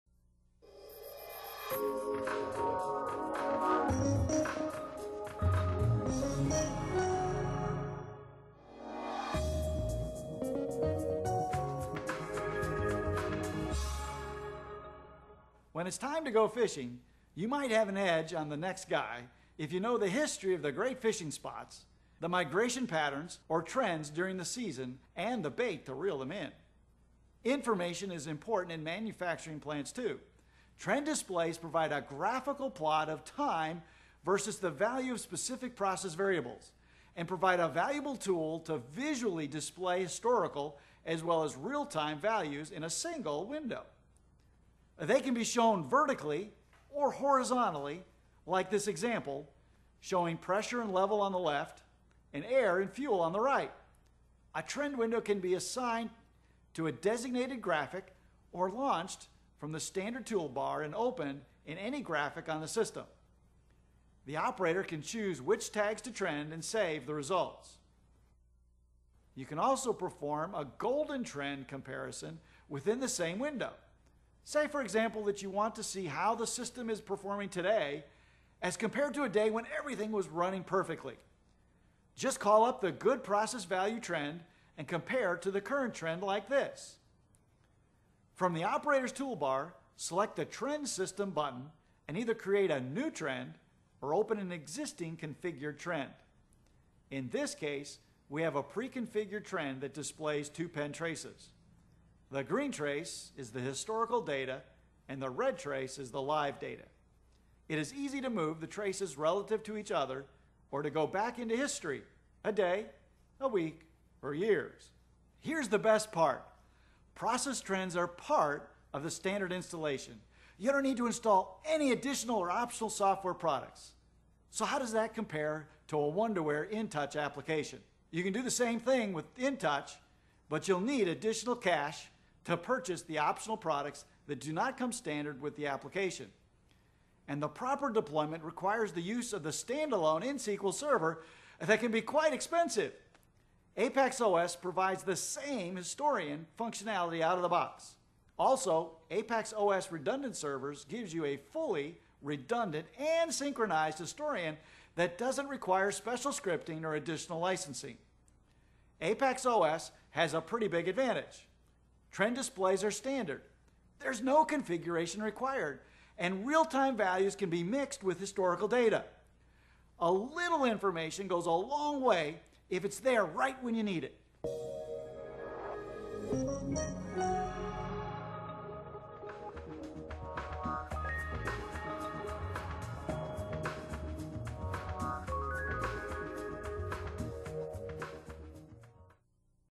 Process trends in APACS+ OS vs. Wonderware InTouch method
This video shows out-of-the-box process trends in APACS+ OS and compares them with the Wonderware InTouch method. There is also a how-to that shows you how to compare a current trend to a historical trend of the same variable.